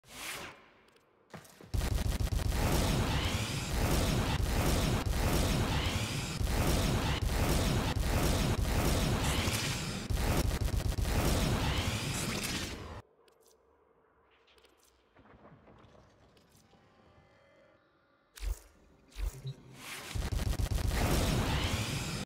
The Main Reason I NEVER Play Volt
Maybe your thinking I get annoyed by the sound his speed makes. Sorry but thats a hard nope. Its because every relic mission every shorty. All random pubs do is spam 2. Like they are trying to speed run. Even see it once in awhile on onslaught level farming. Volt Prime believe it or not is really strong but yet people just spam his 2. He is used in eidolons for damage. I don't hate the frame I just don't agree with how people use him in normal missions. Also my color scheme for volt prime at the time was inspired by an old cartoon use to watch Static Shock another flash like character. Pretty cool show. But ya spamming speed annoys me probably why I play solo most of the time lol.